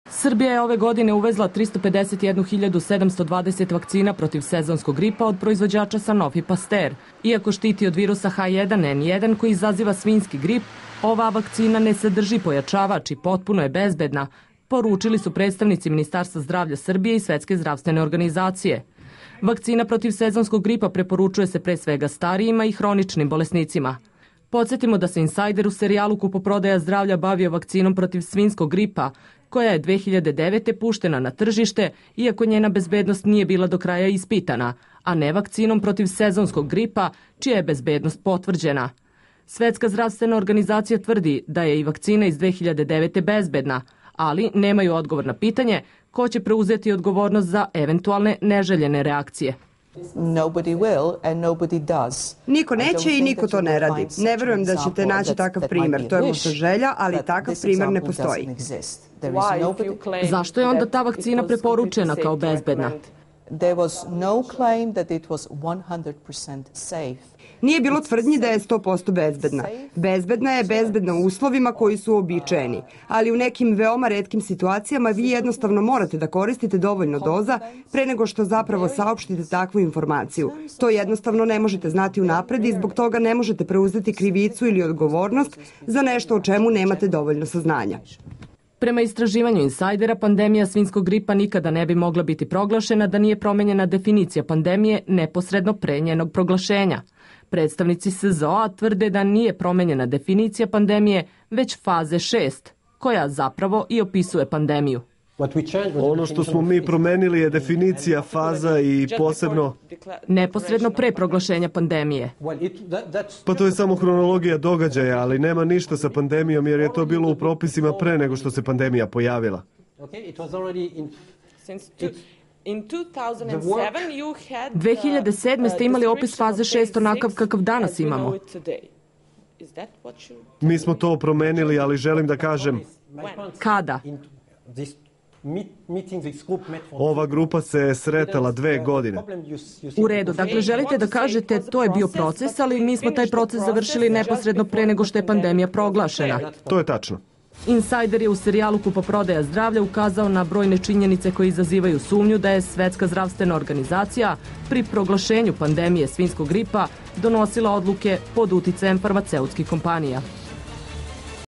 Vakcina protiv gripa je bezbedna
Srbija ove godine uvezla 351.720 vakcina protiv sezonskog gripa od
proizvodjaca Sanofi Paster. U Ministarstvu zrdavlja i SZO tvrde da je
vakcina bezbedna. Oni su takodje porucili da vakcina ne sadrzi pojacavac.
Istovremeno, stiti od virusa H1N1 koji izaziva i svinjski grip.